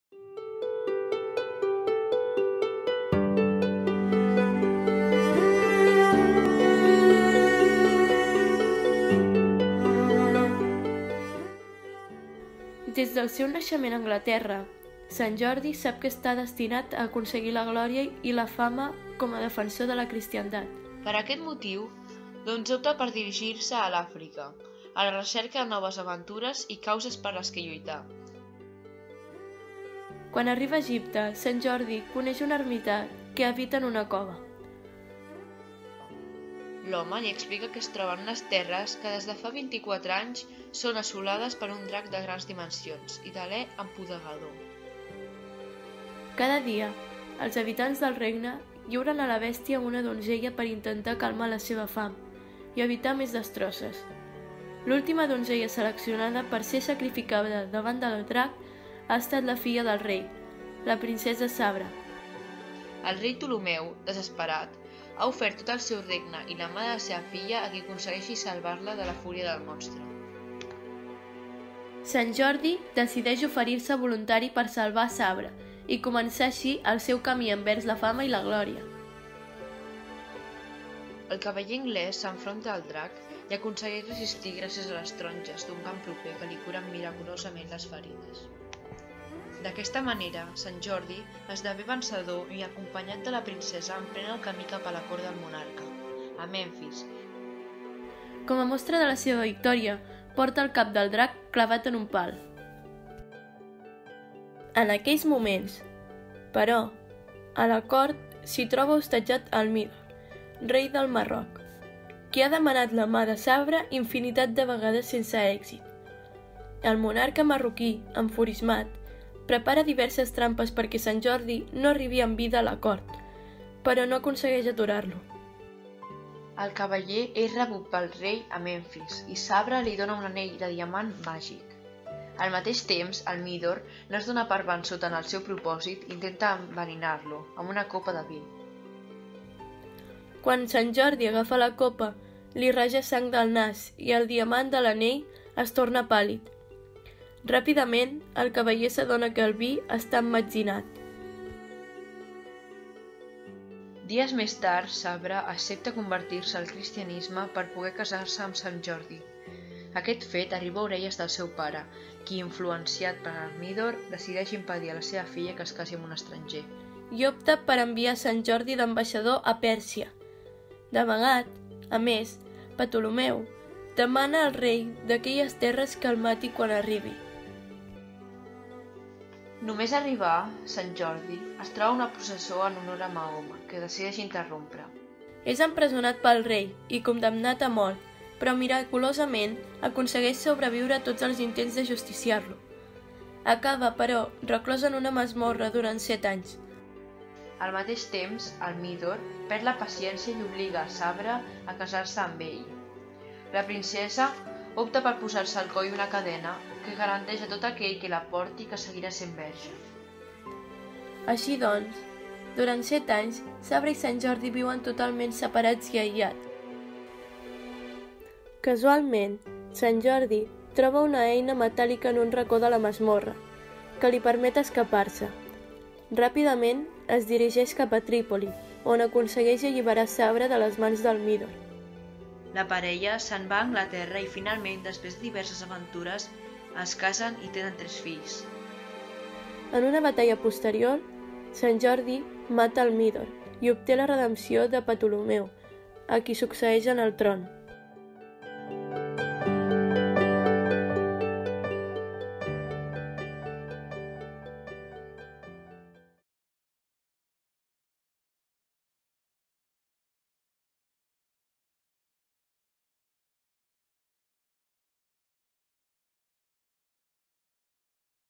La llegenda de Sant Jordi en el paravent del Palau Güell
El Servei de Patrimoni Arquitectònic Local de la Diputació de Barcelona us presenta la versió anglesa força desconeguda a Catalunya de la llegenda de Sant Jordi, que va ser escrita el 1596 per Richard Johnson. Aquest autor, contemporani i influenciat per William Shakespeare, es va inspirar en el romanç anglès de Sir Bevis of Hampton, del segle XIII,, i va incloure el text en la seva obra titulada The Most Famous History of the Seven Champions of Christendom [La història més famosa dels set defensors de la cristiandat], on es bateja la princesa de sant Jordi amb el nom de Sabra.
Aquesta denominació, orientalitzada com a Sahabra, va ser utilitzada per l’artista Alexandre de Riquer per esmentar el personatge en els vitralls dedicats a la llegenda que conformen un paravent de fusta i aplicacions metàl·liques conservat al Palau Güell de Barcelona i que han estat estudiats des del punt de vista iconogràfic pel Servei.
La narració de la llegenda està il·lustrada amb imatges evocadores dels seus diferents passatges on podreu descobrir el paravent i la residència barcelonina d’Eusebi Güell i Bacigalupi, del qual aquest any 2018 se celebra el centenari de la seva mort i que va ser projectada per l’arquitecte Antoni Gaudí: un Palau de conte, un Palau per a la imaginació.